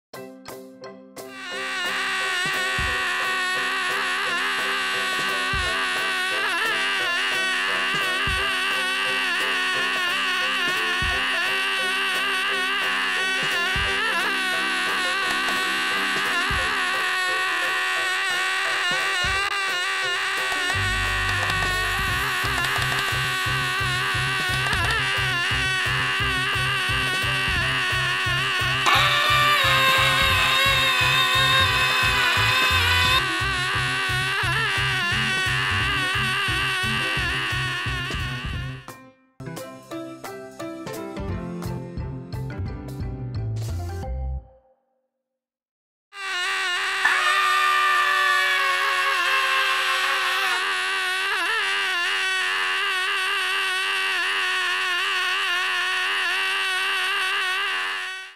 Rick and Morty Screaming Sun Alarm Clock from ThinkGeek
-Rick & Morty Screaming Sun Alarm Clock
-Officially-licensed Rick and Morty merchandise
-A ThinkGeek creation & exclusive
-Sun raises up over the mountains and screams when it's time to wake up!
-Set your alarm to be greeted by 42 consecutive hours of that beautiful melody
-Just like the sun on that planet from "The Wedding Squanchers"
-Volume dial lets you set it anywhere between 80 and 90 dB
-Materials: Plastic
-Imported
-Batteries: 2 AAA (not included - can also be powered by included AC adapter)
-Dimensions: 7" tall (with sun up) x 8" wide x 3" deep
-Includes Screaming Sun Alarm Clock and 100-240V 50/60HZ AC adapter with Type A plug